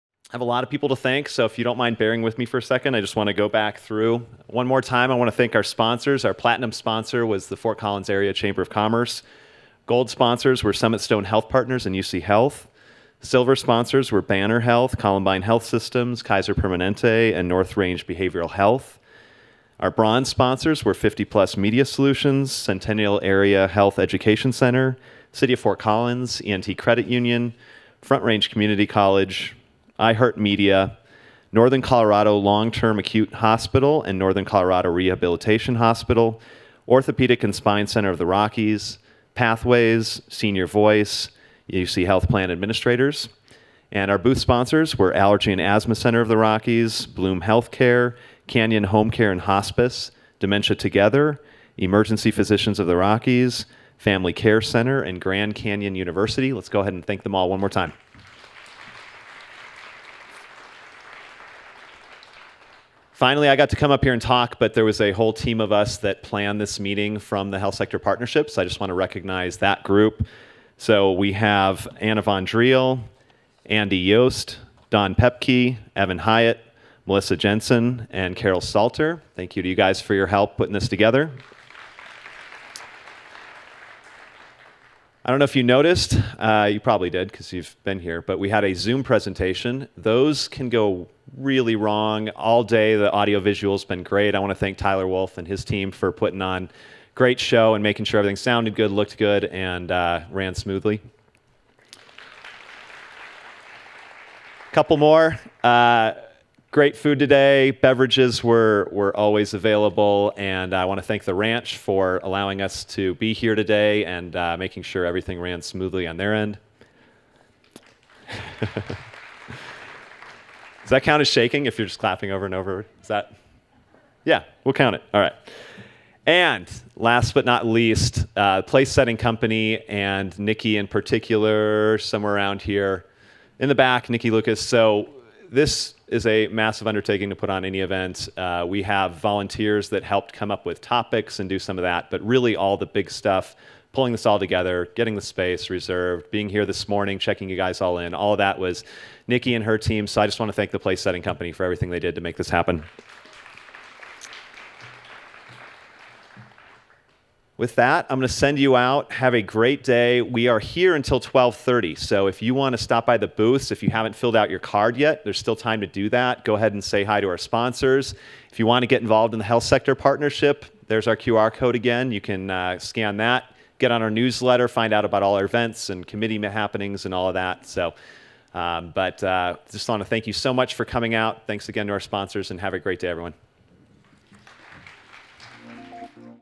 Healthcare in your Future Summit 2023 - Event Close and Thanks
The Health Summit took place on November 1, 2023 at The Ranch Events Complex in Loveland, Colorado.

Hosted by the Northern Colorado Health Sector Partnership, the event aims to provide members of the business community the opportunity to discuss developments in health care, an outlook on the healthcare industry in the region, and actions that could strengthen our local healthcare sector.